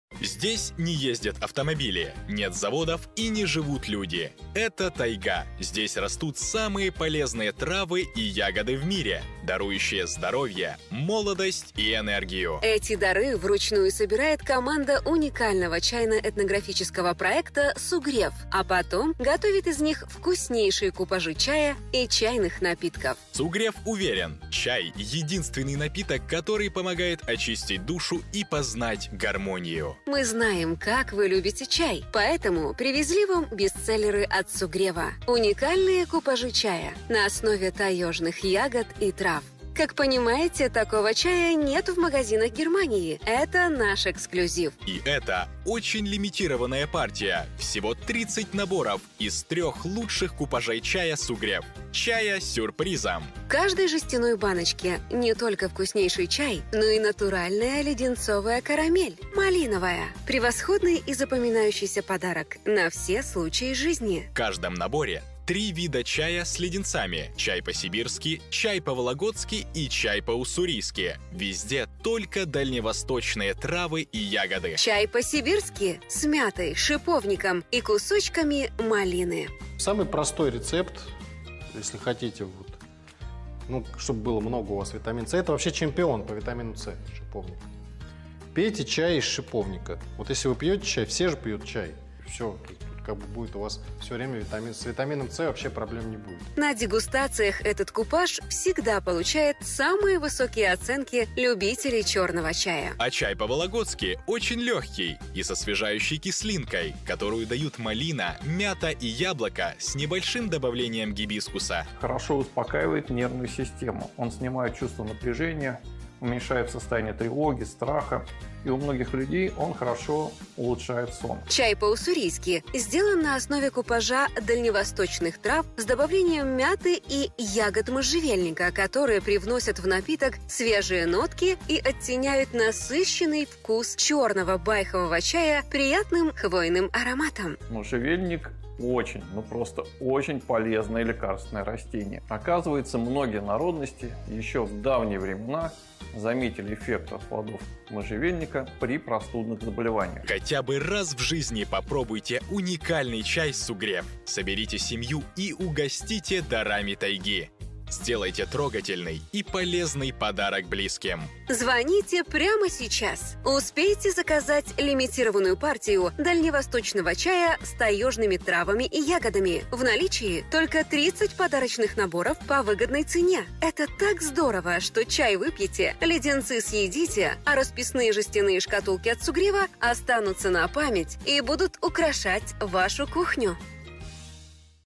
Подарочный чайный набор Sugreff: 3 вида чая со сладостями (3x90 г)
hier bestellen:
Подарочный чайный набор Sugreff: три чайных набора с сибирскими , уссурийскими и вологодскими конфетами (3 х 90 г)
Чайный набор "Sugréff" с конфетами по-сибирски с клюквой и малиной, 40+50 г:Чайный набор "СИБИРСКИЙ" - это отличный и незабываемый подарок на все случаи жизни. Он состоит из уникального купажа чая и 100% натуральных конфет со вкусом малины. Чай "СИБИРЬЯН" изготовлен на основе купажа натуральных таежных трав и ягод с добавлением мяты, шиповника и кусочков малины, которые подчеркивают вкус длиннолистового черного чая с приятной ягодной свежестью. На дегустациях этот купаж неизменно получает высокие оценки любителей черного чая.
Состав чая: черный индийский чай Ассам 51%, шиповник 10%, цветки пиона 9%, мята 5%, клюква 10%, кусочки малины 8%, 40 г.
Состав карамельных конфет со вкусом малины: сахар, патока, вода, регуляторы кислотности (лимонная кислота, молочная кислота), натуральные ароматизаторы, красители: концентрат сладкого картофеля, редиса и яблока, 50 г.
Чайный набор "Sugréff" с конфетами в уссурийском стиле с можжевельником и мятой, 40+50 г: Чайный набор "Уссурийский" - это отличный и незабываемый подарок на все случаи жизни. Он состоит из уникального купажа чая и 100% натуральных конфет со вкусом малины. Чай "Уссурийский" изготовлен на основе смеси натуральных дальневосточных трав с добавлением мяты и ягод можжевельника, которые придают напитку свежие нотки и подчеркивают насыщенный вкус черного длиннолистового чая с приятной сладостью - кисловато-хвойным ароматом.
Состав чая: черный индийский чай Ассам, ягоды можжевельника, мята, душица, клюква, 40 г.
Составкарамельных конфет со вкусом малины: сахар, патока, вода, регуляторы кислотности (лимонная кислота, молочная кислота), натуральные ароматизаторы, красители: концентрат сладкого картофеля, редьки и яблока, 50 г
"Sugréff" Чайный набор с конфетами "Вологодский стиль" с малиной и красной смородиной, 40+50 г:Чайный набор "Вологодский" - это отличный и незабываемый подарок на все случаи жизни. Он состоит из уникального купажа фруктово-травяного чайного напитка и 100% натуральных конфет со вкусом малины. Чайный напиток "Вологда" изготовлен из смеси натуральных трав и фруктов. Это легкий, приятный напиток с освежающей кислинкой, которую придают малина, мята и яблоко с нотками гибискуса. Особенно в теплое время года или в душных помещениях зимой такой чай очень приятно пить.
Состав чая: гибискус 20%, яблоко 15%, ромашка 15%, мята 15%, малина 15%, календула 15%, лист черной смородины, 40 г.
Ингредиентыкарамельные конфеты со вкусом малины: сахар, патока, вода, регуляторы кислотности (лимонная кислота, молочная кислота), натуральные ароматизаторы, красители: концентрат сладкого картофеля, редиса и яблока, 50 г